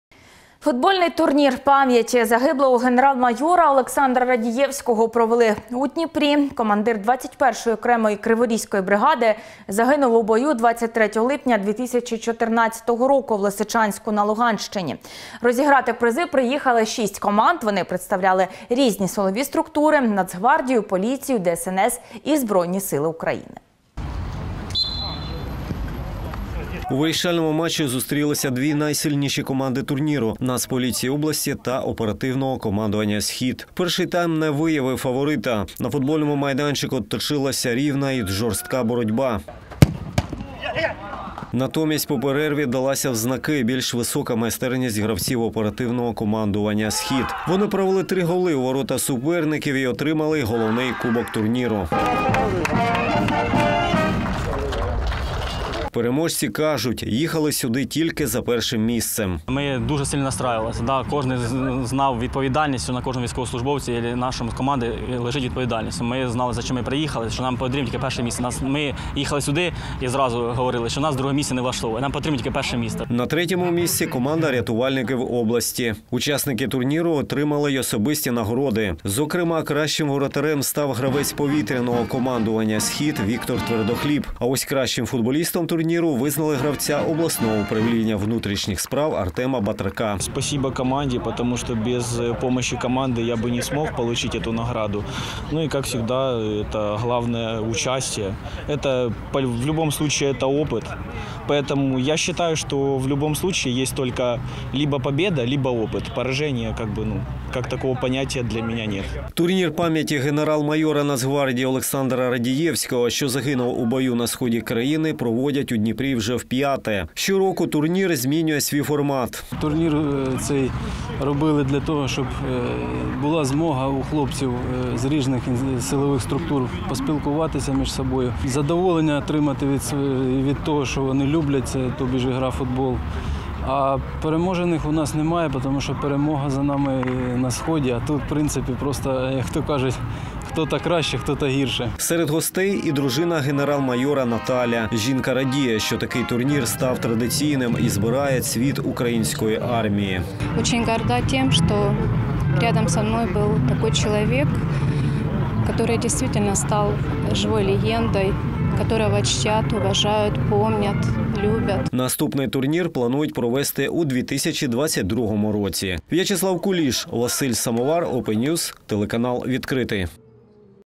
У Дніпрі силовики зіграли у футбол
Футбольний турнір пам’яті загиблого генерал-майора Олександра Радієвського провели у Дніпрі. Командир 21-ї окремої криворізької бригади  загинув у бою 23 липня 2014 року в Лисичанську, що на Луганщині. Розіграти призи приїхали 6 команд. Вони представляли різні силові структури - Нацгвардію, поліцію, ДСНС і Збройні сили України.